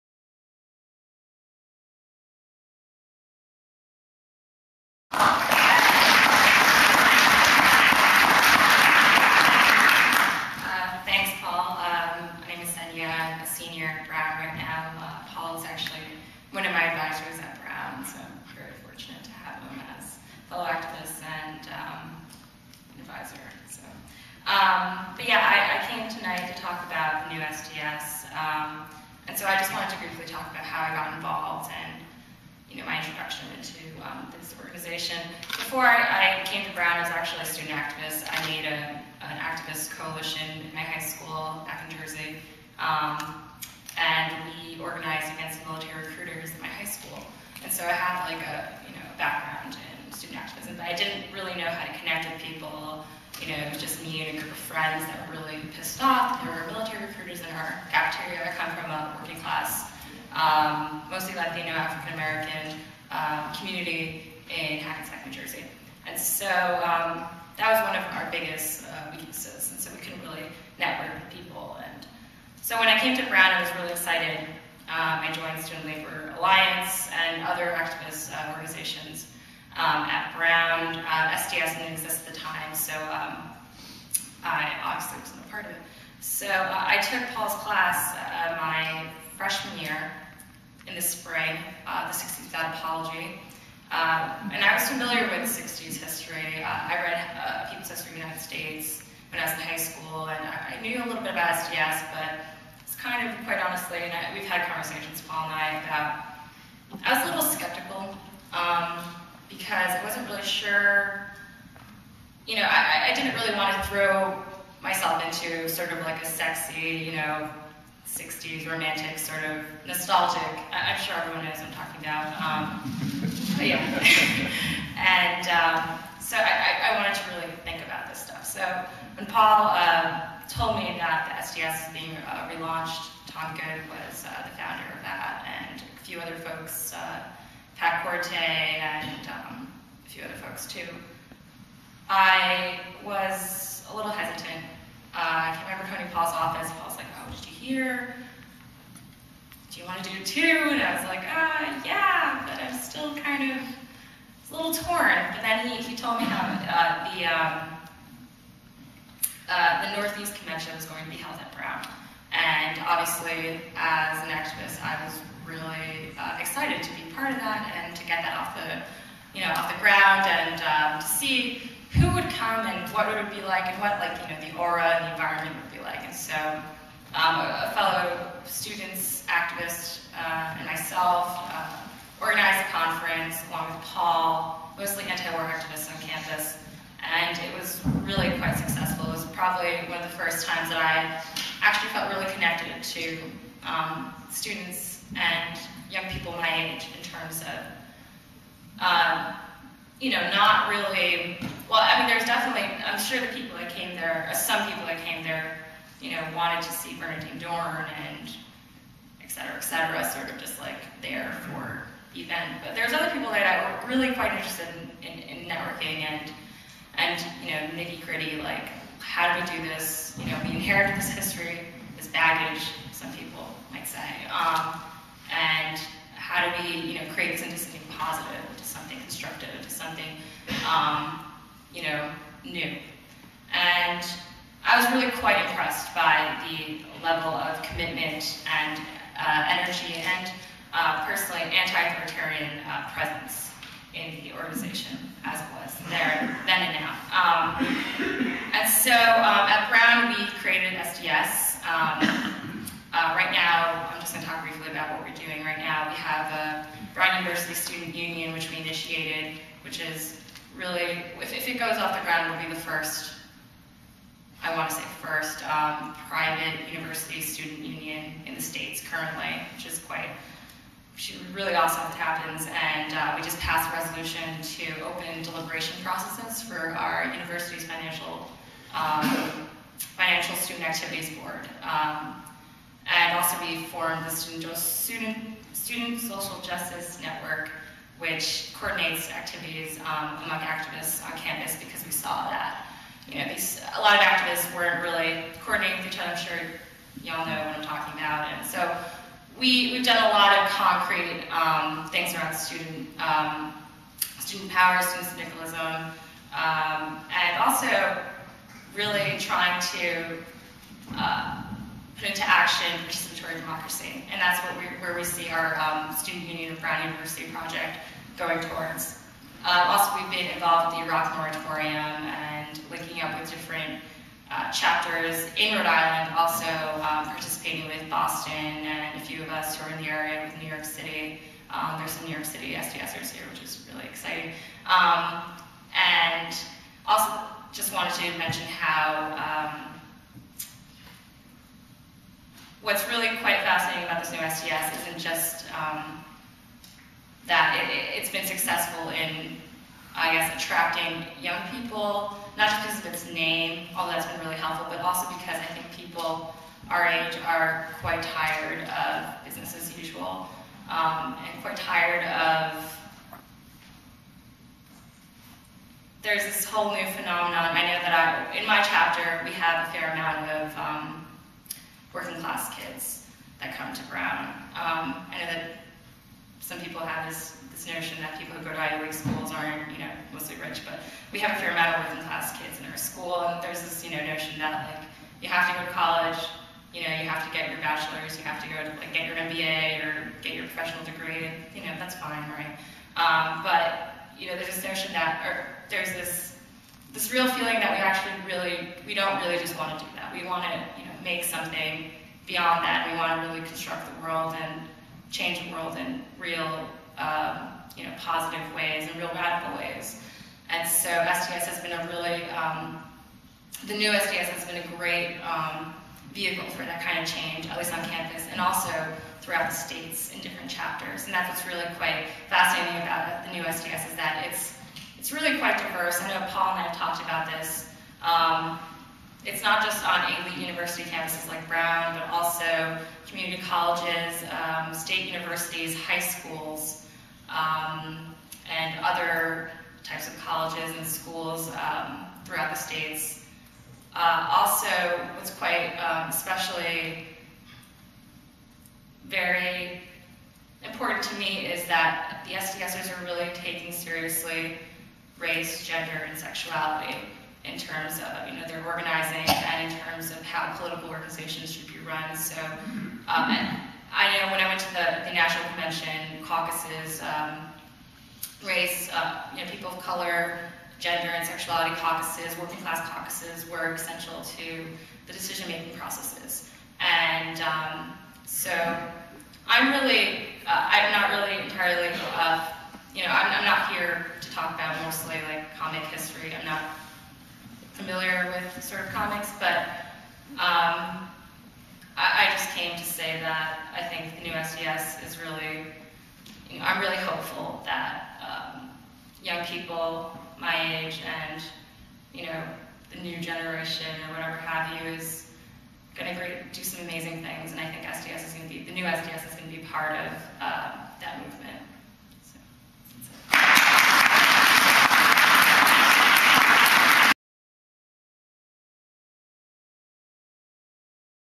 SDS Comic Roadshow - Senia Barragan
December 10, 2007 - New York, NY. The City University of New York (CUNY) Graduate Center on Fifth Avenue was the site of a Students for a Democratic Society (SDS) "comic roadshow". Editor Paul Buhle, writer Harvey Pekar and cartoonist/activist Nick Torkelson joined with Jeff Jones, former SDS/Weather Underground leader and Senia Barragan, new SDS member from the Brown Univ chapter to promote "SDS: A Graphic History". In this clip, Senia Barragan discusses new SDS.